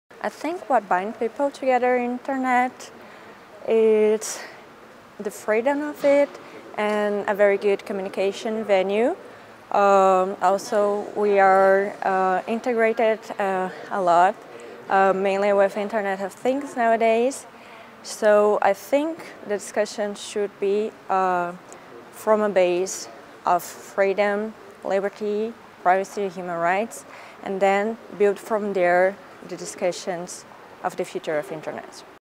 Global IGF 2019: Bruna Toso de Alcântara on Enhancing Digital Cooperation
Bruna Toso de Alcântara, student, Federal University of Rio Grande do Sul, Brazil, shares their answer to the question, “What will bring people together and build better cooperation globally on digital issues, digital regulation, security and trust?” Recorded at the Global Internet Governance Forum 2019 in Berlin, Germany, by the Imagining the Internet Center, an initiative of Elon University, North Carolina, USA.